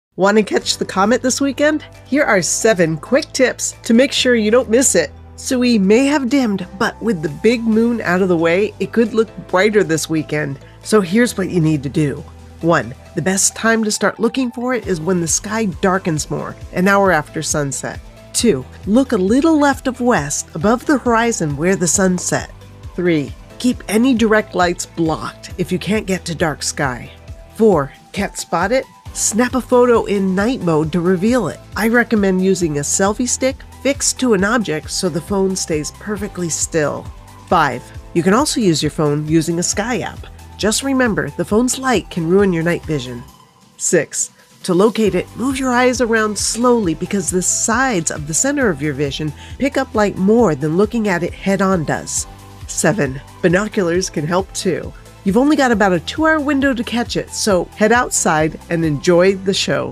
How to see the Comet October 19th ☄️ Tsuchinshan-ATLAS C/2023 A3
Want to catch the comet this weekend? Comet Tsuchinshan-ATLAS C/2023 A3may have dimmed, but with the moon out of the way, it could shine brighter! In this video, I’ll share 7 quick tips to help you spot it, from the best time to look, where to aim your gaze, and even how to use your phone or binoculars to make sure you don’t miss out.